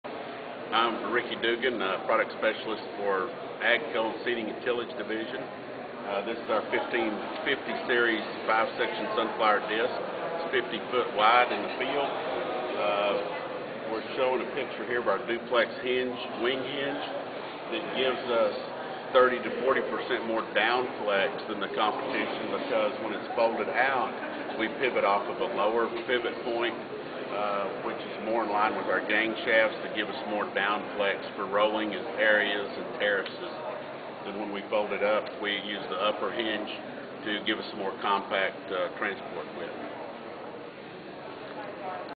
AGCO Features on the 1550 Series Disc Harrow For Tillage.
The 1550-Series, five-section disc harrow features duplex wing hinges and walking triple tandems, never seen on tillage equipment before. Here what Ricky Dugan, Product Specialist for AGCO's High-Horsepower Tractors, Tillage and Seeding Equipment, has to say.